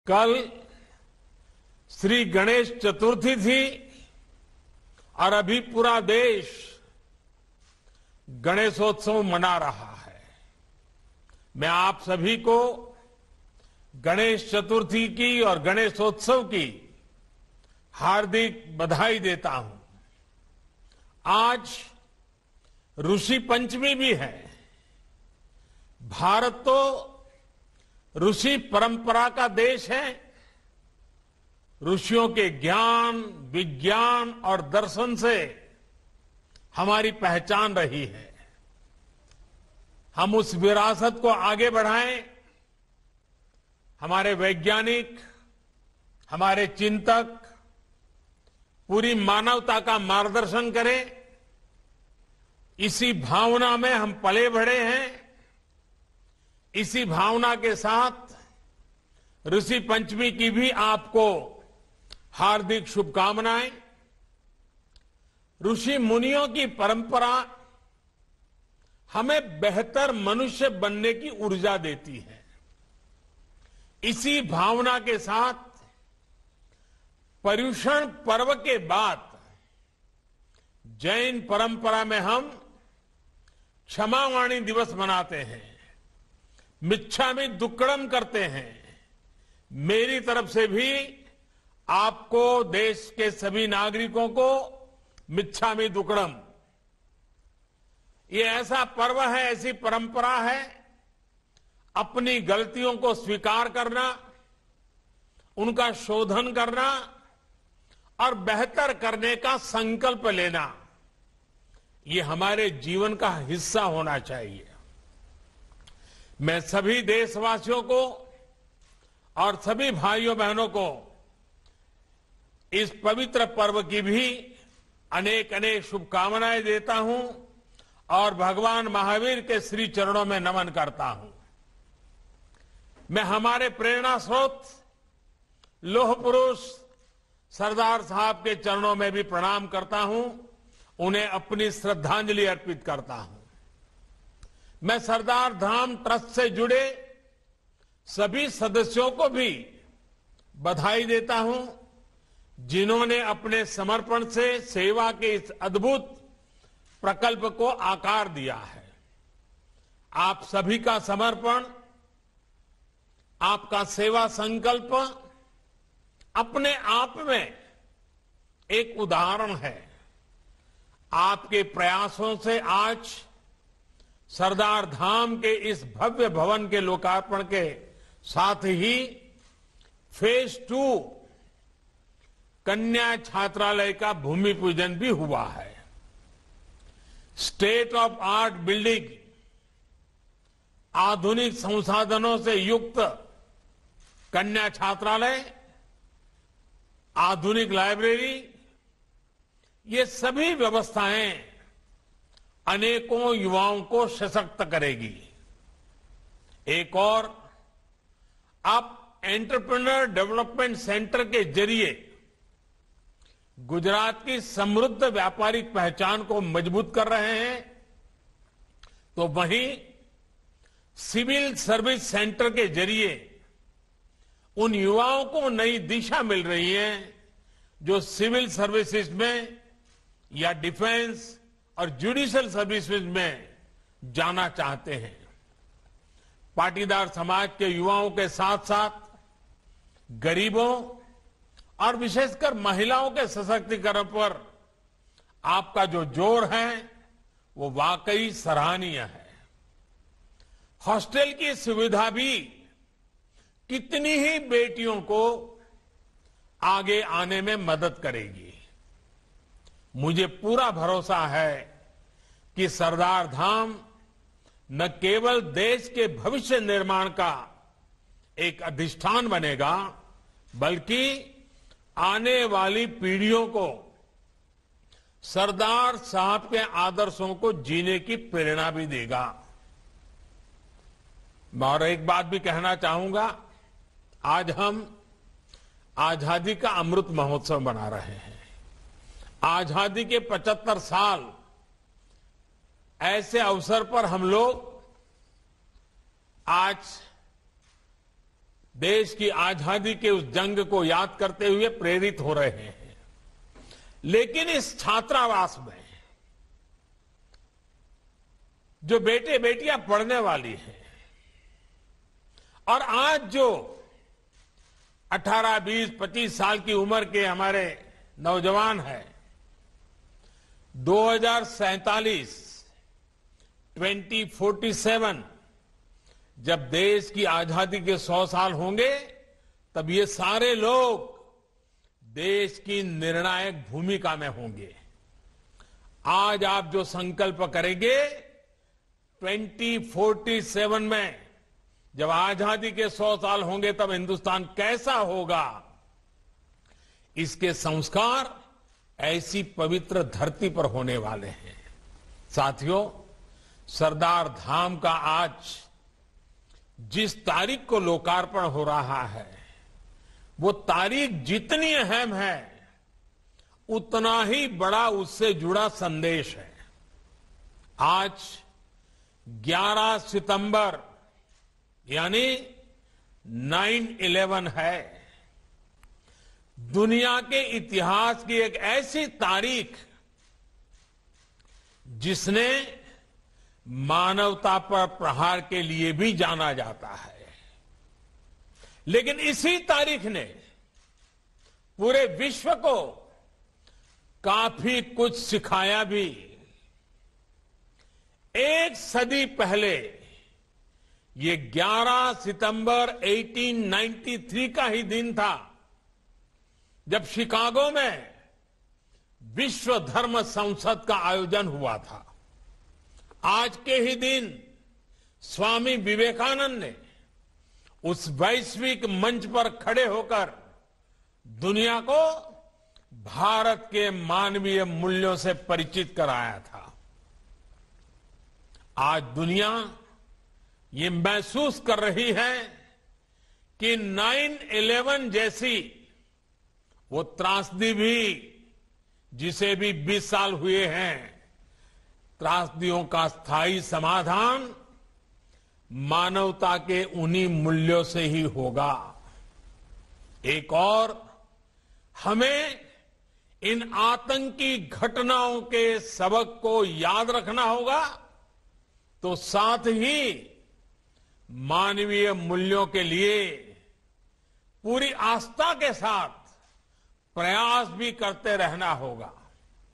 9/11 की 20वीं बरसी पर बोले PM Modi - इस तारीख ने पूरे विश्व को काफी कुछ सिखाया है
Pm Narendra Modi ने शनिवार को अहमदाबाद में सरदारधाम भवन (Sardardham Bhavan) का लोकार्पण किया. यह भवन बेहतर नौकरी की इच्छा रखने वाले ग्रामीण क्षेत्रों के लड़कियों और लड़कों को छात्रावास की सुविधा मुहैया करवाएगा. पाटीदार समाज द्वारा बनाया गया यह कॉम्प्लेक्स छात्रों को उचित दर पर ट्रेनिंग, बोर्डिंग, लॉज की सुविधाएं प्रदान करेगा. लोकार्पण कार्यक्रम में पीएम मोदी ने अमेरिका में हुए 9/11 आतंकी हमले का भी जिक्र किया.